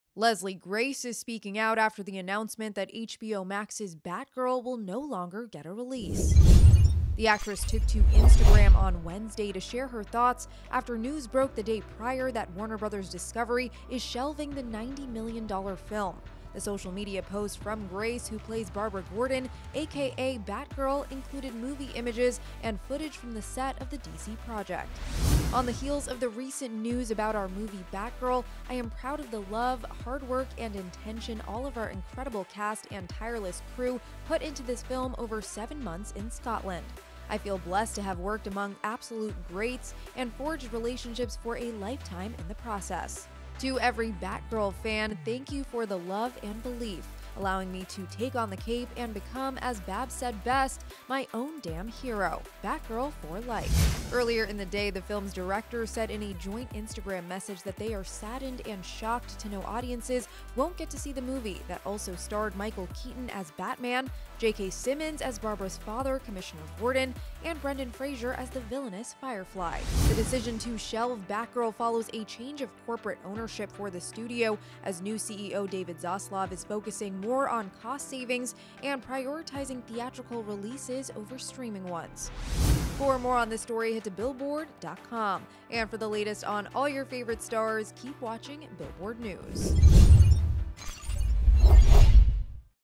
‘Batgirl’ Star Leslie Grace Responds After Film Is Shelved | Billboard News
The actress took to Instagram to share her thoughts after news broke the day prior that Warner Bros. Discovery is shelving the $90 million film.